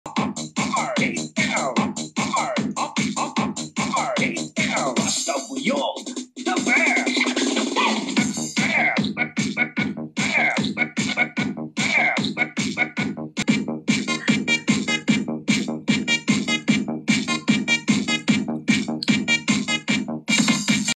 Chinese woman loves showing of her freakishly long tongue by knitting a necklace
开州幺妹（小号） Kaizhou Yaomei (trumpet) Douyin live stream clip